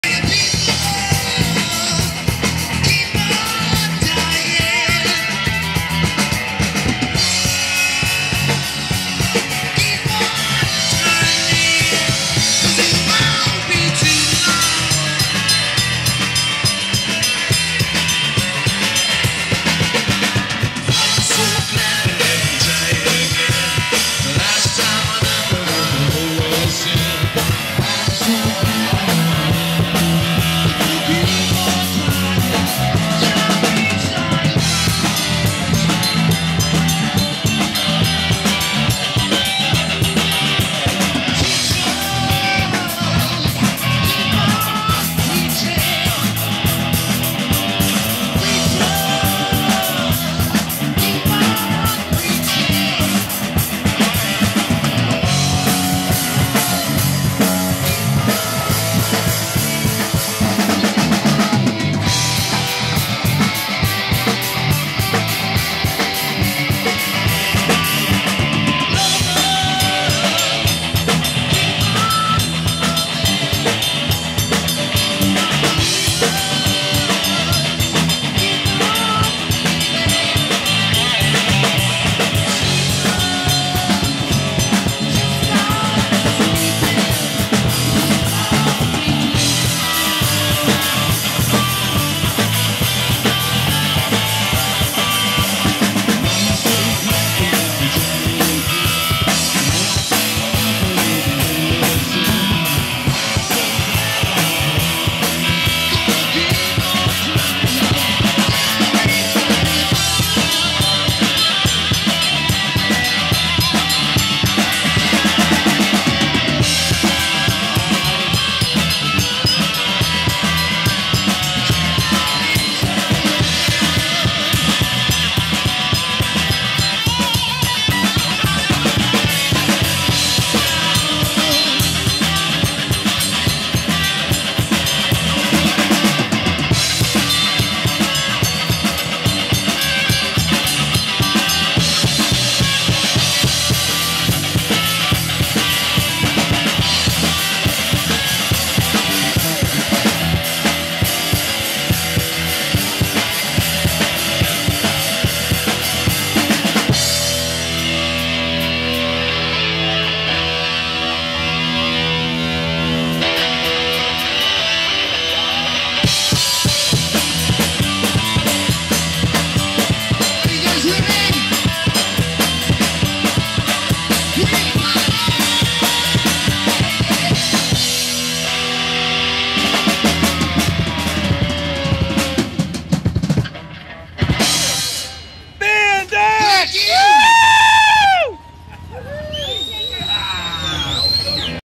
BAND X - Strawberry, AZ 2015
This is BAND X performing at the Rim Country District of the Modified Motorcycle Association's Bikini Bike Wash fundraiser on June 27, 2015